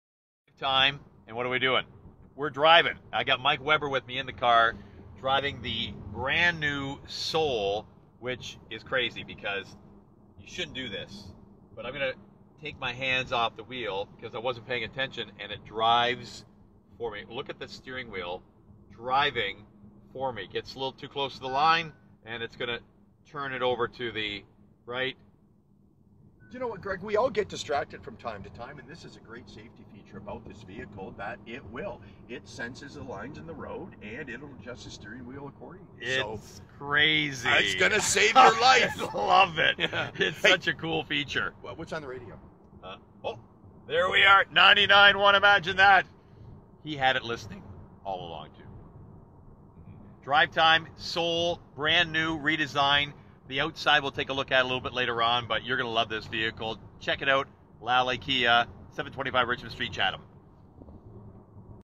Lally Kia
We do some great drives with Lally Kia on the morning show each day this week. I had a chance to drive the 'All new Soul' With Lally Kia's Mike Weber.